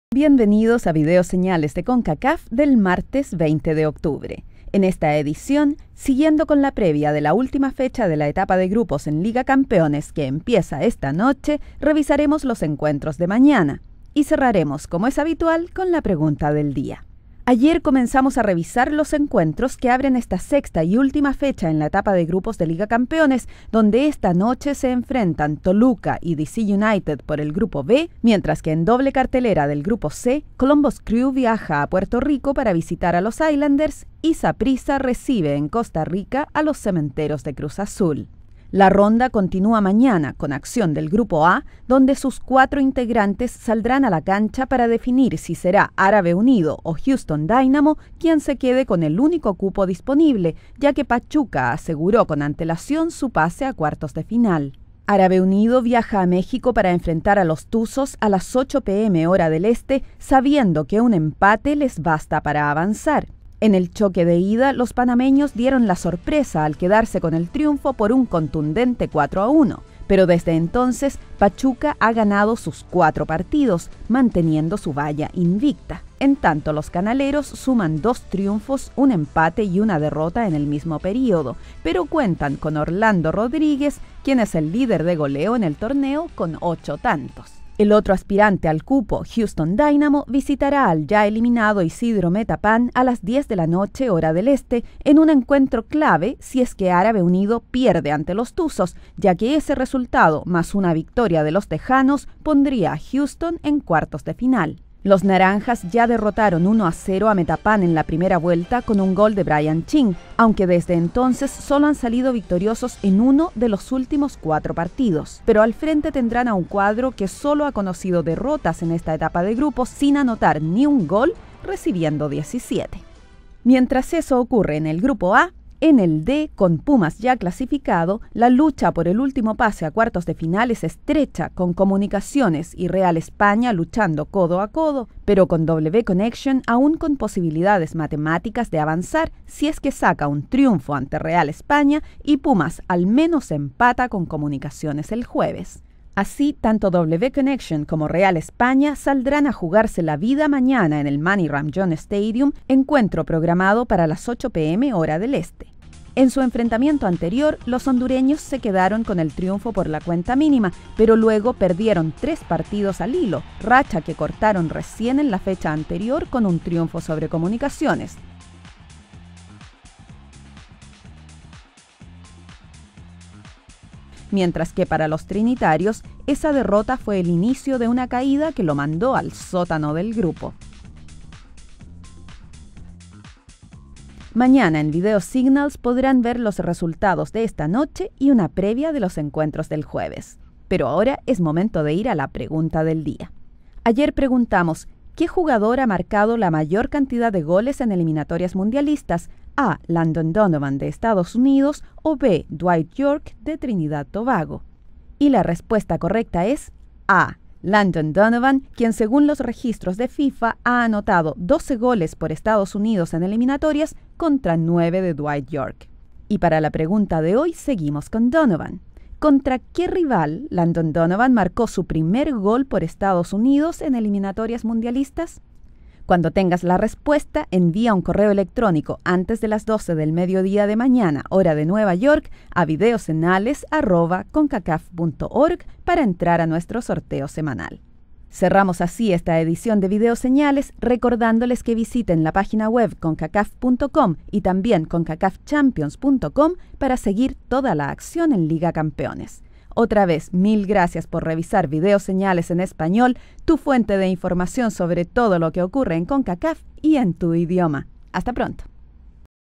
VIDEO SENALES CONCACAF 10/20/09
Video Noticias de Futbol Concacaf, Martes 20 de Octubre, 2009. Incluye previa de los partidos del Miercoles 21 por la Sexta fecha de la Etapa de Grupos en Liga Campeones.